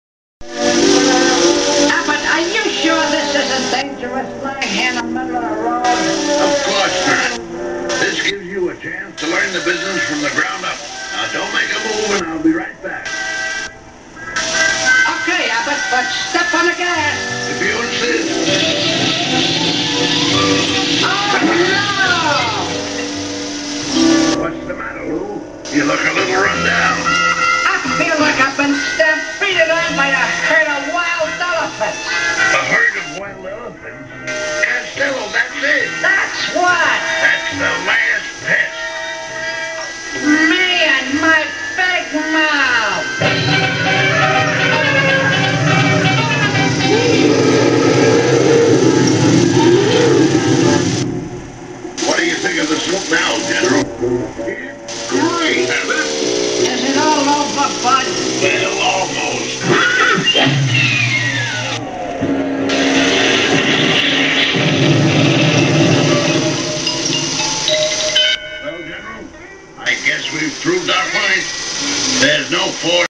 NATCO 16mm projector
This is a quick video of my NATCO Model 3030 16mm movie projector. Production date was about 1950. It is unusual in that it is a "Lefty" projector. It is a nice running projector with good sound. Lighting in the basement was lousey for filming this and I shot it with regular photo camera, not a movie camera, so apologies for the poor quality.